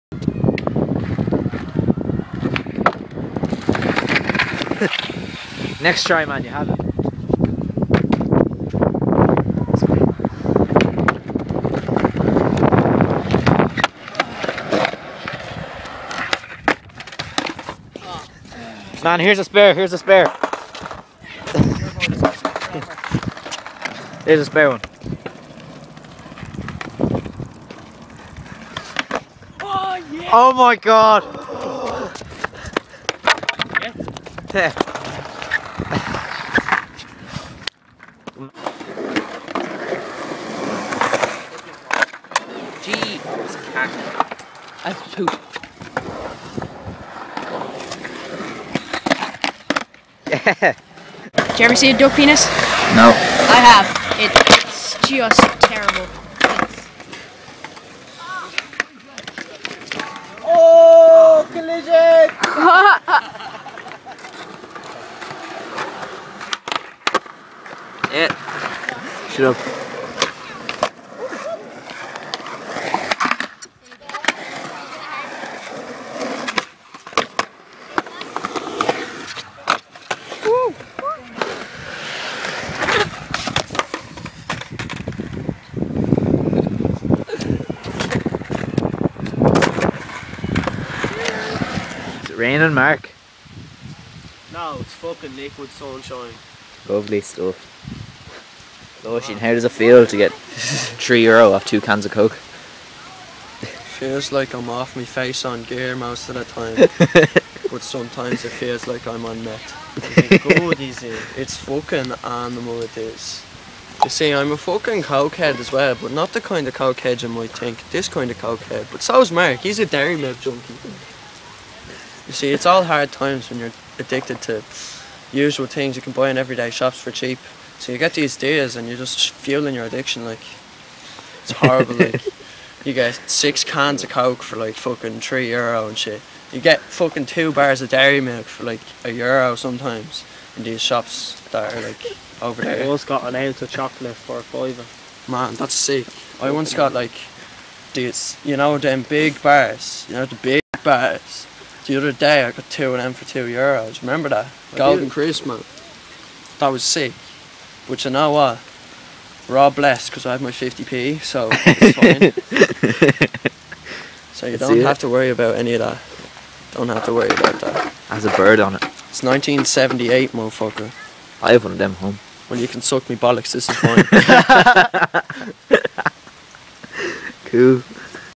INTENSE iPOD SKATEBOARD SHRED TAGE #1
these highly defined intense top notch skateboarding clips of very difficult manouvers were all filmed and edited on an ipod touch, there may be more i havent desided if we are gnarly enough to make another one yet.